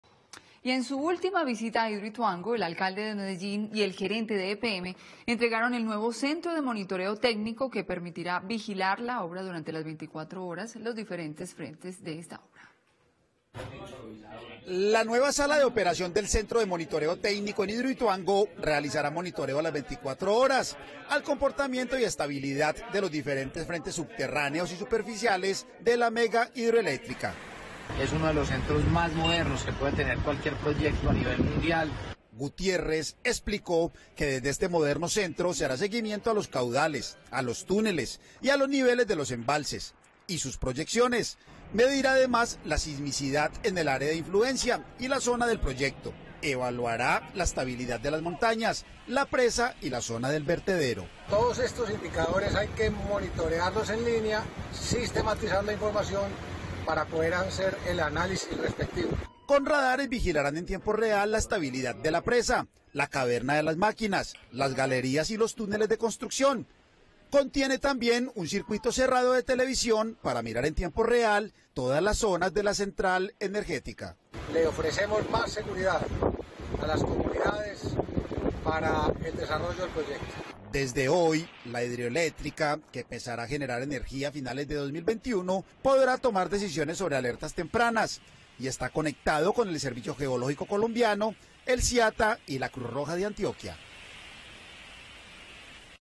EPM estrena nuevo Centro de Monitoreo Técnico en Hidroituango [Noticias] - Telemedellín
En la última visita a Hidroituango, el Alcalde de Medellín y Gerente de EPM, entregaron el nuevo Centro de Monitoreo Técnico.